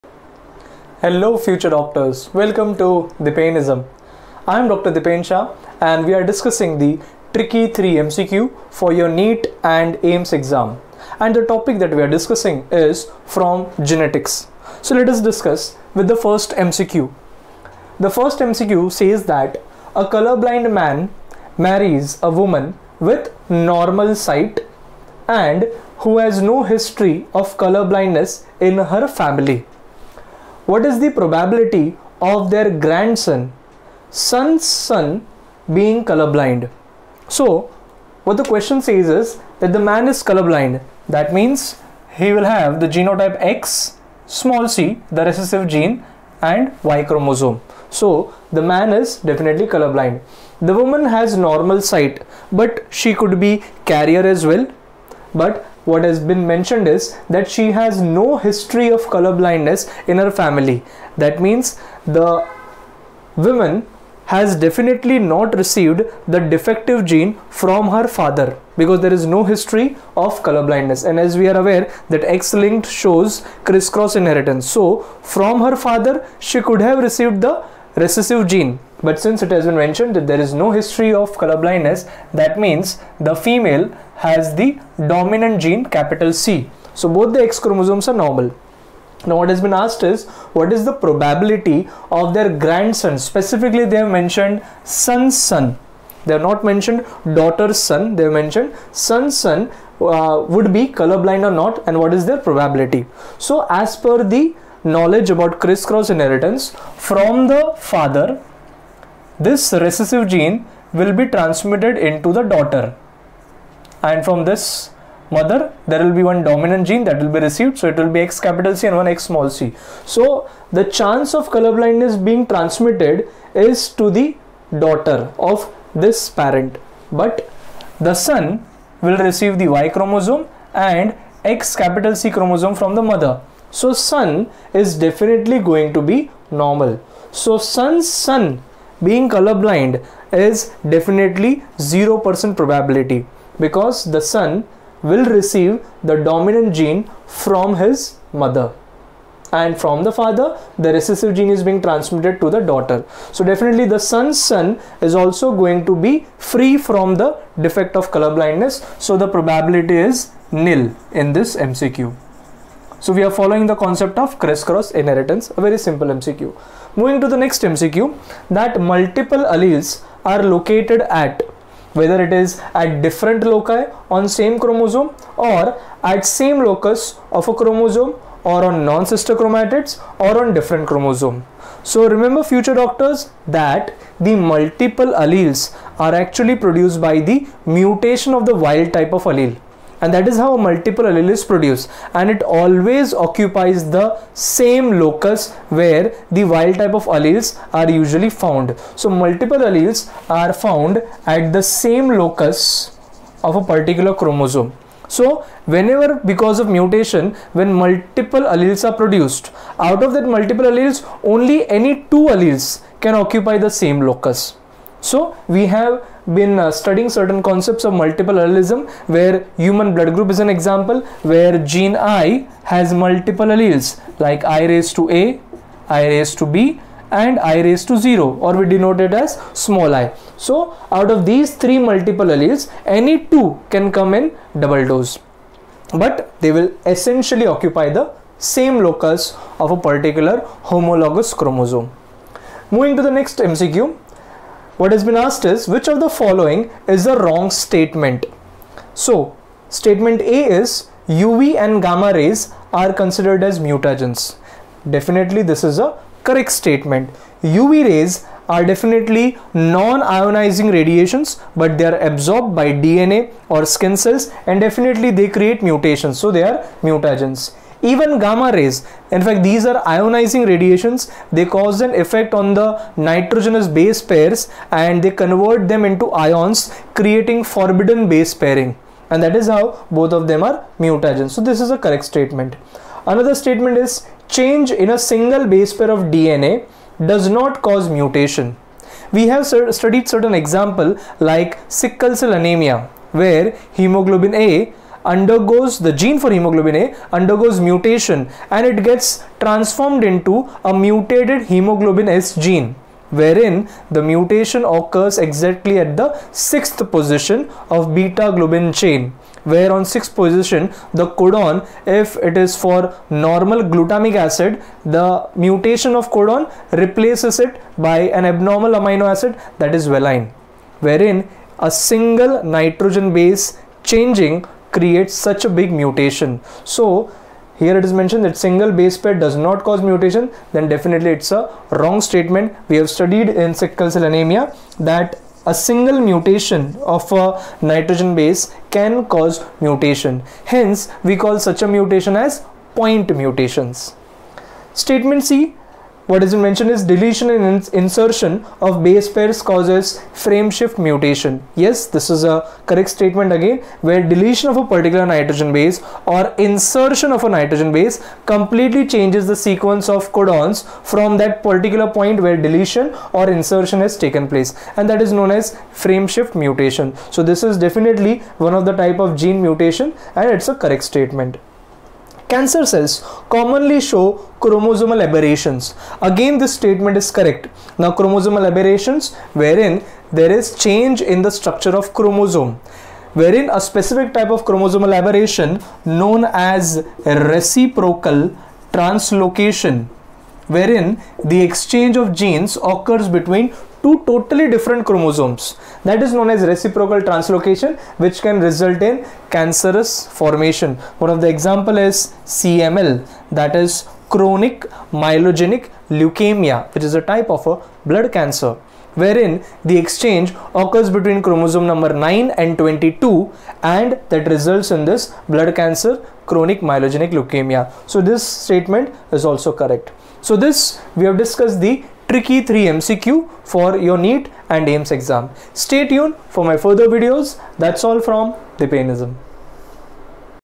Genetics | NEET BIOLOGY
Rapid Tricky Three MCQ Discussion from Genetics

Specialized Coaching for Pre Medical Entrance exams NEET/ AIIMS

Visit website for Downloading free NEET practice papers with Solutions